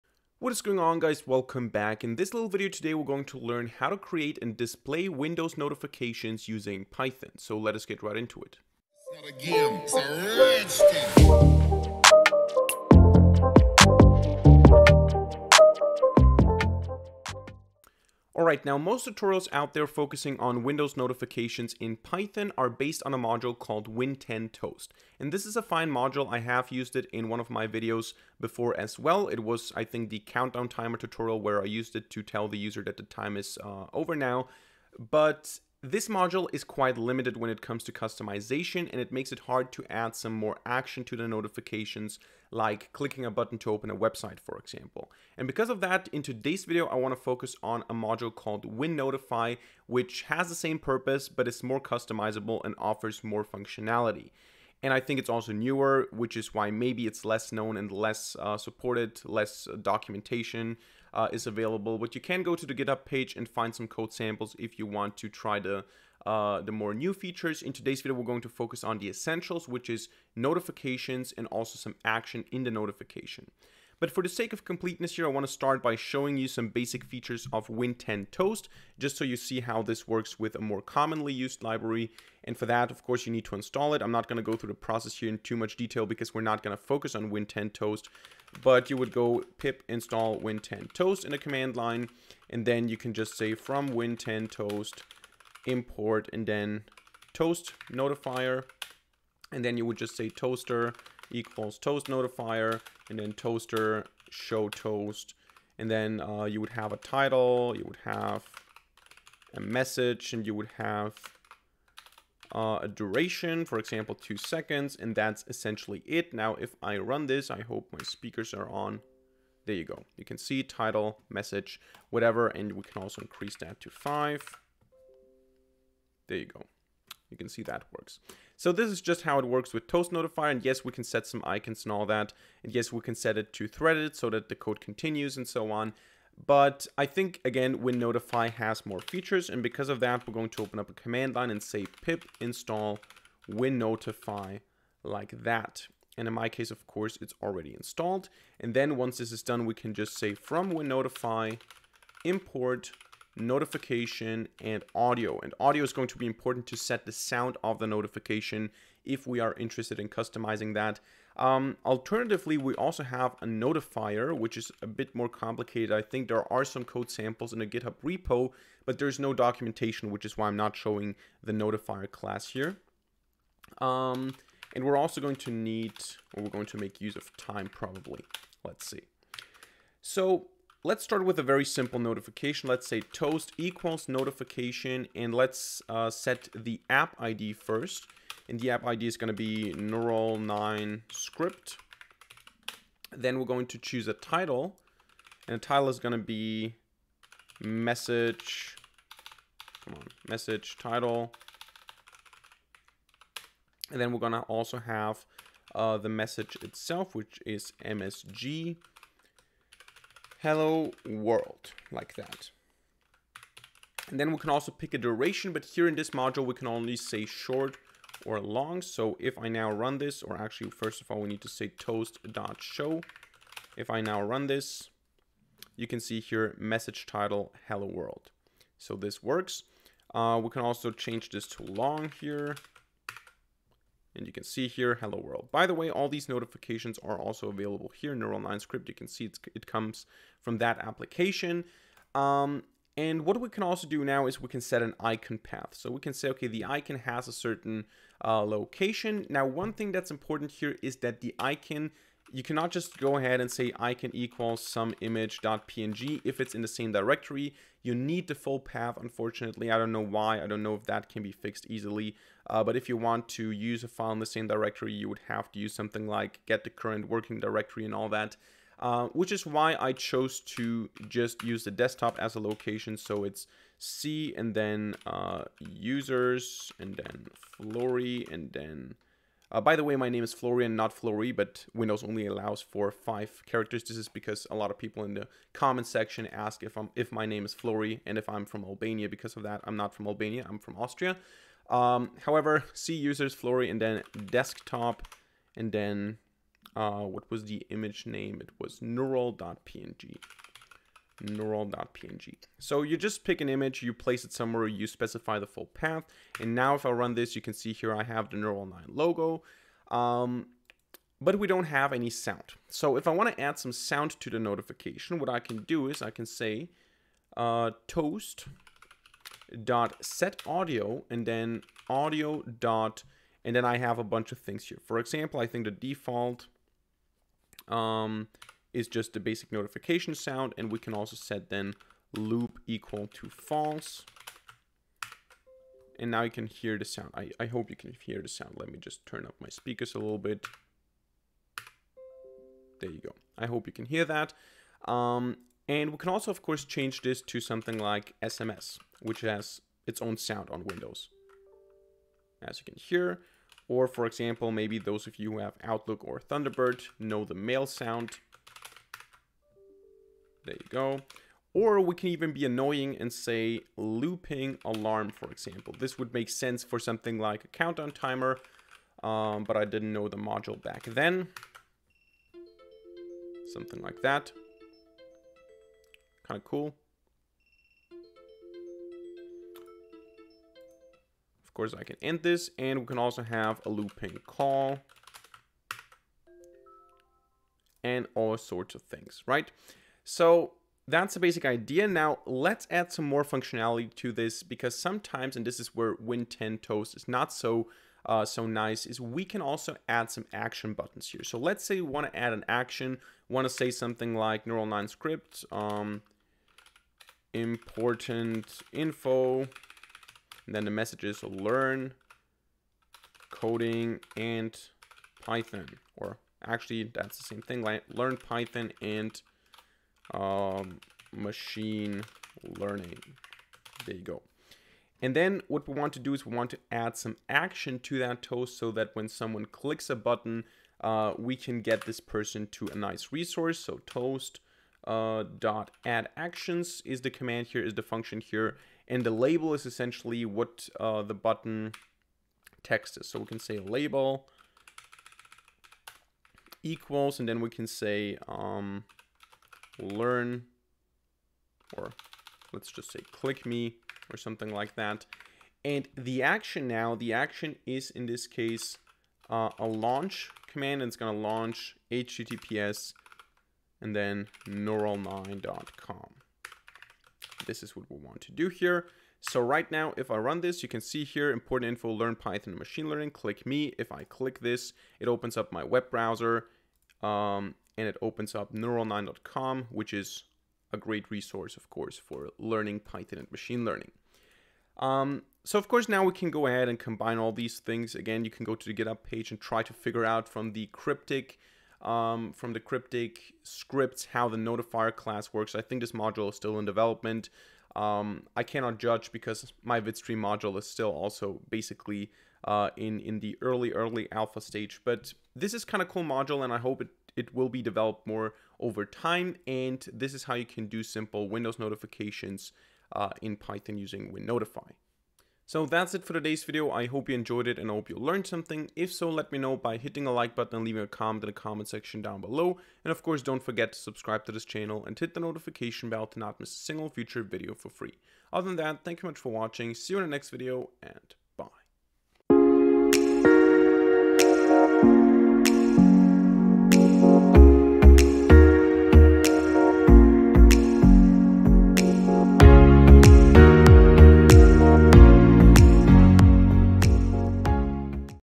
Windows Notifications in Python
Today we will learn how to create and display Windows notifications in Python.